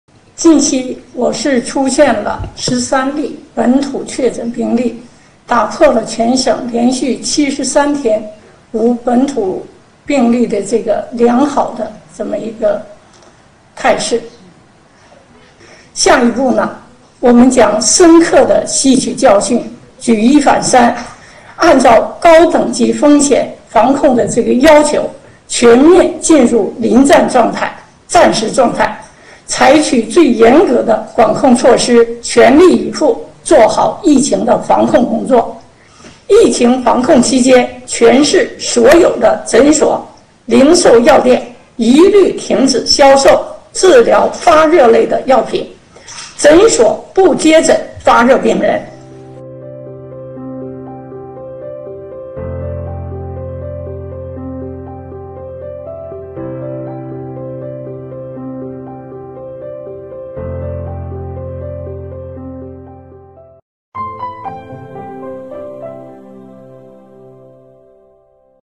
吉林舒兰进入“战时”状态 为中国唯一高风险地区
5月11日，吉林省舒兰市市长金华称，近期，舒兰市出现了13例本土确诊病例，舒兰市将全面进入战时状态。疫情防控期间，全市所有诊所、零售药店一律停止销售治疗发热类药品，诊所不接诊发热病人。目前，舒兰市为全国唯一高风险地区。